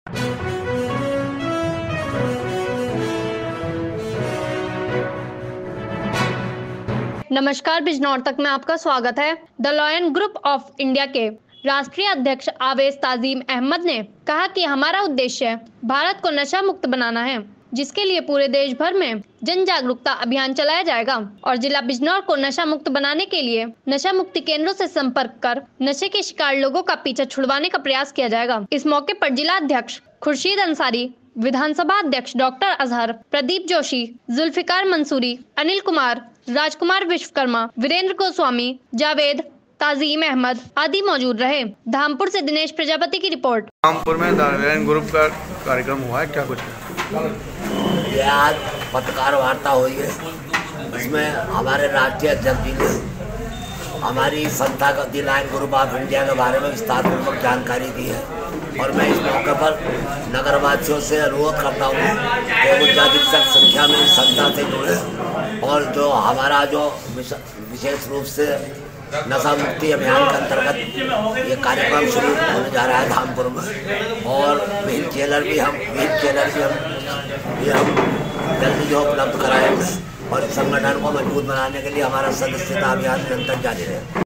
BIJNOR TAK || धामपुर-हमारा उद्देश्य भारत देश को नशा मुक्त बनाना-अहमद
धामपुर। द लॉयन ग्रुप ऑफ इंडिया के राष्ट्रीय अध्यक्ष आवेज तांजीम अहमद ने कहा कि हमारा उद्देश्य भारत देश को नशा मुक्त बनाना है। जिसके लिए पूरे देशभर में जन जागरूकता अभियान चलाया जाएगा और जिला बिजनौर को नशा मुक्त बनाने के लिए नशा मुक्ति केन्द्रों से सम्पर्क कर नशे के शिकार लोगों का पीछा छुड़वाने का प्रयास किया जाएगा।
स्थानीय महल सराय स्थित डा.अजहर के क्लीनिक पर पत्रकारों से वार्ता के दौरान राष्ट्रीय अध्यक्ष आवेज तांजीम अंसारी ने कहा कि उनका संगठन स्वास्थ्य और शिक्षा के क्षेत्र में जरूरतमंदों की सेवा करना चाहता है। वह हर नगर में मौहल्ला पाठशालाओं का आयोजन करेंगे। जिसमें जरूरतमंद परिवारों के गरीब मेधावी छात्र-छात्राओं को हाईस्कूल और इंटर के साथ-साथ एस.एस.सी. की निःशुल्क में कोचिंग देने का काम किया जाएगा। पहला प्रयास है कि संगठन बिजनौर जिले में पहले धामपुर से शुरुआत होगा। उन्होंने जनता को साथ लेकर नशा मुक्ति कार्यक्रम के प्रति लोगों में जागरूकता लाने का काम शुरू कर दिया है। उन्होंने बताया कि संगठन ने महाराष्ट्र में करीब 70 से अधिक एक ग्रेड के अस्पतालों को संबद्ध कर रखा है। जहां पर मरीजों का निःशुल्क उपचार किया जाता है। उन्होंने बताया कि हमने जिला बिजनौर को नशामुक्त बनाने के लिए हाल ही में एक ज्ञापन उत्तर प्रदेश के मुख्यमंत्री को सम्बोधित ज्ञापन एसडीएम धीरेन्द्र सिंह की गैरमौजूदगी में उनके आशुलिपि को सौंपा था। केवल एक माह के भीतर ही नशा मुक्त भारत के अभियान को बिजनौर जिले में सार्थक बनाने की पहल शुरु हो जाएगी। इस मौके पर जिलाध्यक्ष खुर्शीद अंसारी, विधानसभा अध्यक्ष डा.अजहर, प्रदीप जोशी, जुल्फिकार मंसूरी, अनिल कुमार, राजकुमार विश्वकर्मा, वीरेंद्र गोस्वामी, जाकिर कस्सार, जावेद, ताजीम अहमद आदि मौजूद रहे। धामपुर से दिनेश कुमार प्रजापति की रिपोर्ट